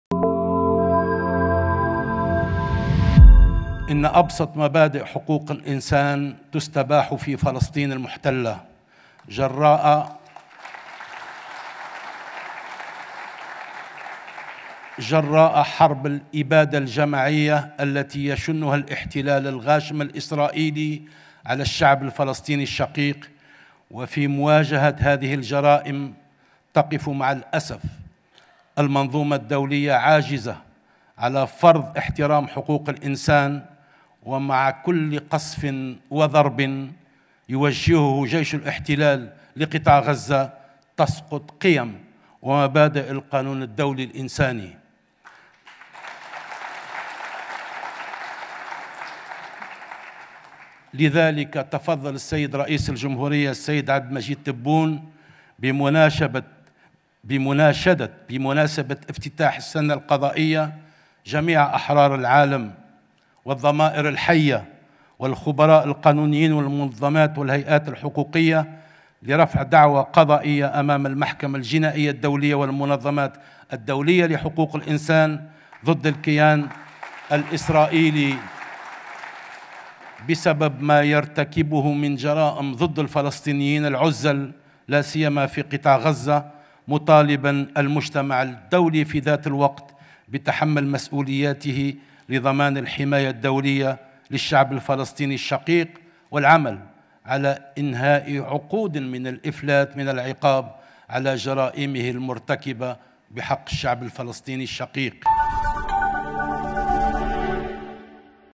العرباوي يجدد دعوة رئيس الجمهورية لمحاكمة الاحتلال الصهيوني على جرائمه ضد الفلسطينيين
الجزائر  (وأج)- جدد الوزير الأول  السيد نذير العرباوي  دعوة رئيس الجمهورية  السيد عبد المجيد تبون  الى جميع أحرار العالم وأصحاب الضمائر الحية والخبراء القانونيين والمنظمات والهيئات الحقوقية لرفع دعوى قضائية أمام المحكمة الجنائية الدولية والمنظمات الدولية لحقوق الإنسان ضد الكيان الصهيوني بسبب ما يرتكبه من جرائم ضد الفلسطينيين  لاسيما في قطاع غزة.